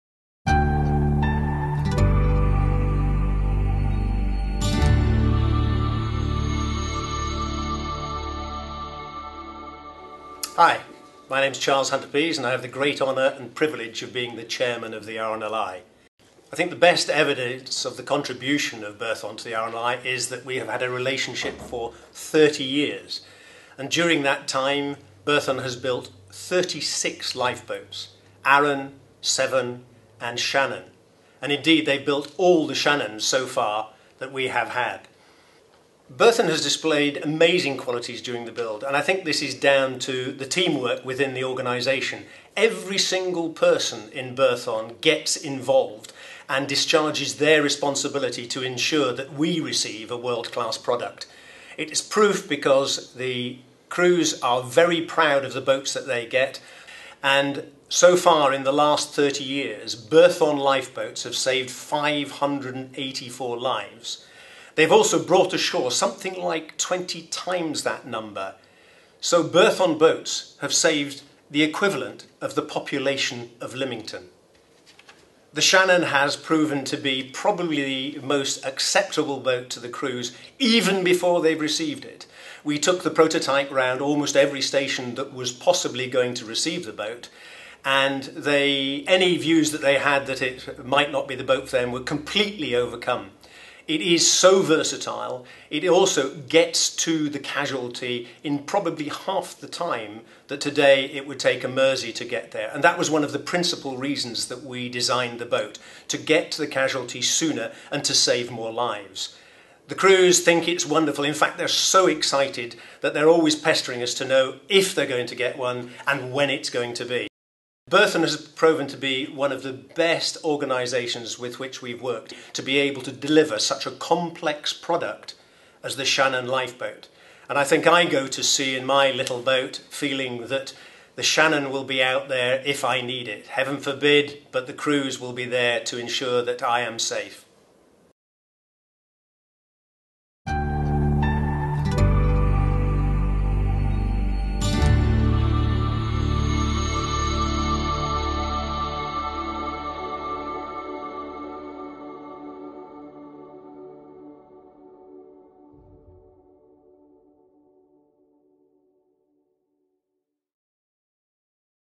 Building the Shannon Class Lifeboat
Charles Hunter-Pease, Chairman of the RNLI, commending Berthon’s boat building abilities.

Berthon built 12 Shannon lifeboats for the RNLI over 3 years. Previously, Berthon built the Arun and Severn class boats with a rich history of building workboats for the Search and Rescue sector.